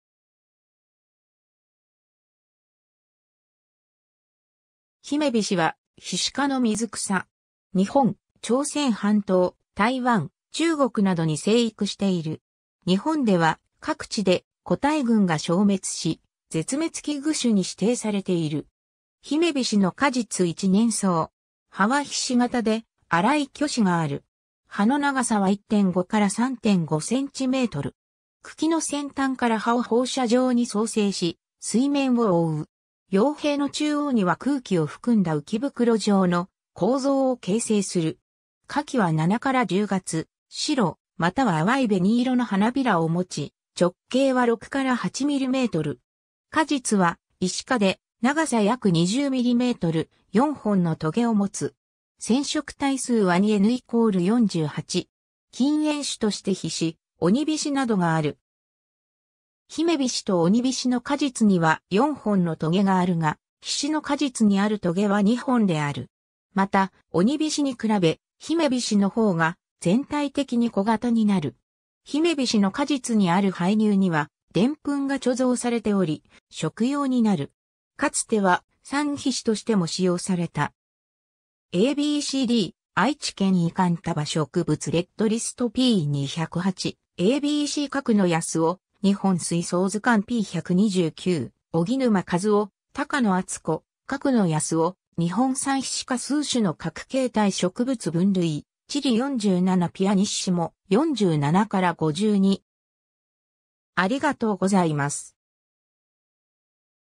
ヒメビシ
ヒメビシ（姫菱 Trapa incisa）は、ヒシ科の水草。
 日本、朝鮮半島、台湾、中国などに生育している。
日本では各地で個体群が消滅し、絶滅危惧種に指定されている。
 ヒメビシの果実 一年草。
葉は菱形で、粗い鋸歯がある。
葉の長さは1.5-3.5cm。
茎の先端から葉を放射状に叢生し、水面を覆う。
葉柄の中央には空気を含んだ浮き袋状の構造を形成する。
 花期は7-10月、白または淡い紅色の花弁をもち、直径は6-8mm。
果実は石果で長さ約20mm、4本のとげをもつ。
 染色体数は2n=48。
 近縁種としてヒシ、オニビシなどがある。
ヒメビシとオニビシの果実には4本のとげがあるが、ヒシの果実にあるとげは2本である。
またオニビシに比べ、ヒメビシの方が全体的に小型になる。
 ヒメビシの果実にある胚乳にはでんぷんが貯蔵されており、食用になる。
 かつては撒菱としても使用された。
 ^ a b c d 「愛知県維管束植物レッドリスト」（2009年）p.208 ^ a b c 角野康郎『日本水草図鑑』（1994年、文一総合出版）p.129 ^ 荻沼一男、高野温子、角野康郎(1996)「日本産ヒシ科数種の核形態」植物分類・地理 47(1) pp.47-52